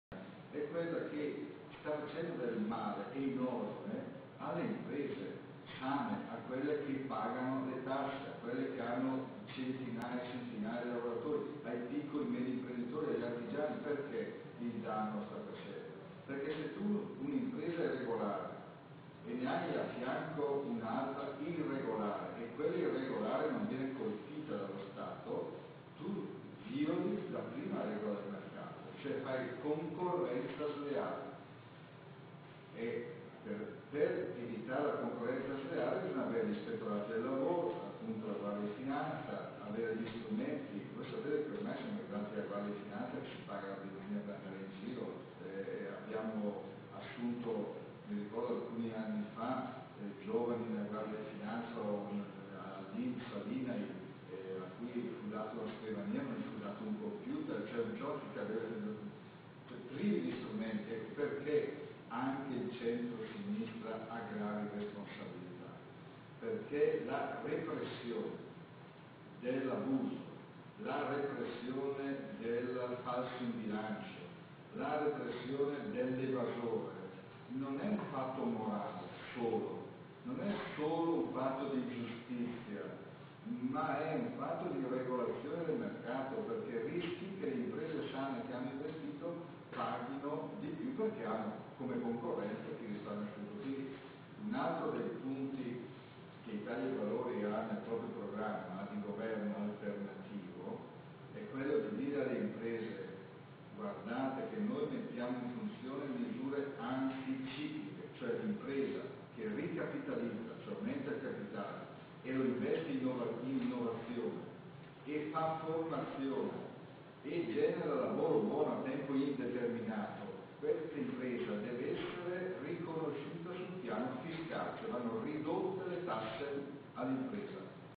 Qik - Zipponi parla dei danni del Governo Berlusconi by staffIdv
Streamed by staffIdv in Italy.